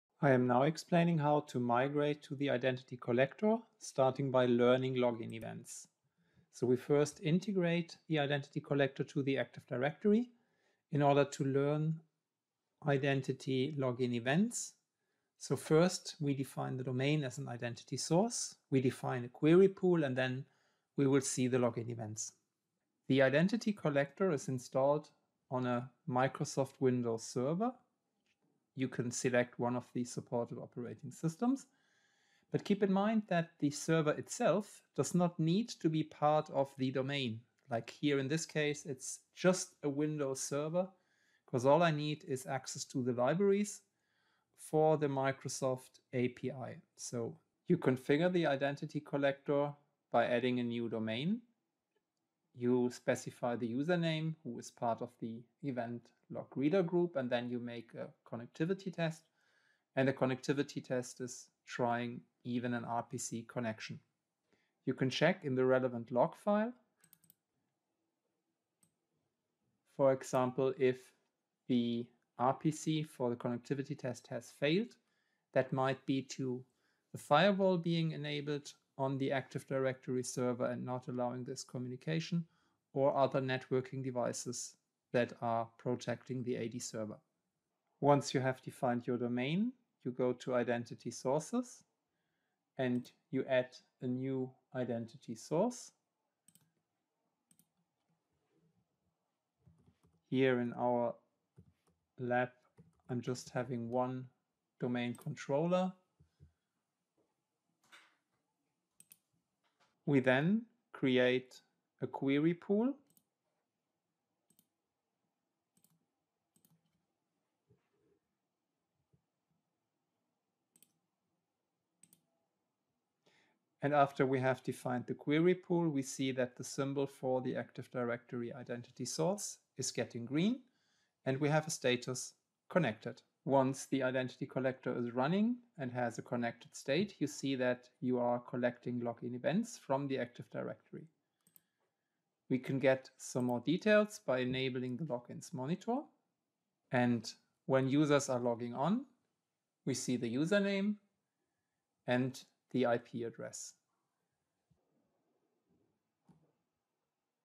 4-Integrating to Identity Collector
Integrating Identity Collector to the Active Directory Domain
Defining Active Directory Domain as an Identity Source
Adding a Query Pool
Monitor Login Events